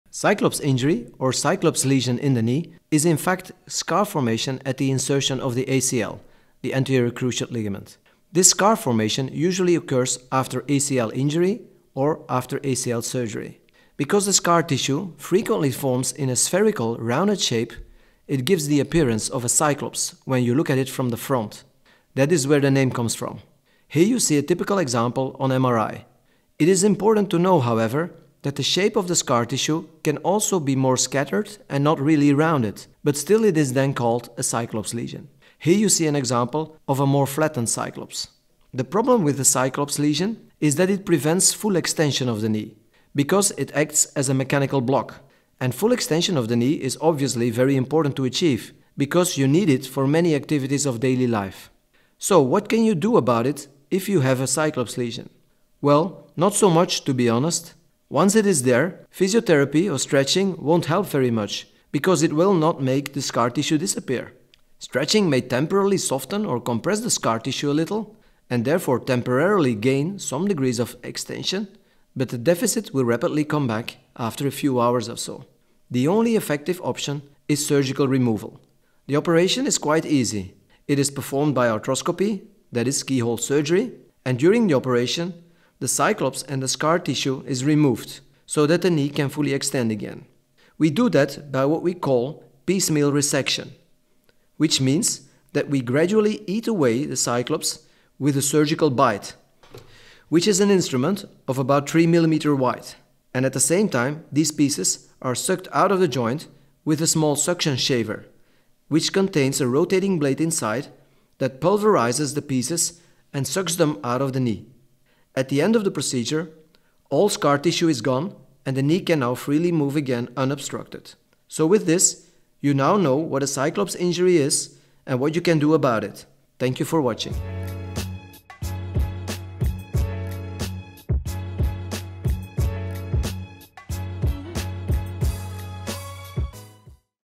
Cyclops injury of the knee -Diagnosis and treatment
In this video Prof. Dr. Bellemans explains what a cyclops injury or lesion of the knee is, how to diagnose it and how you can treat it.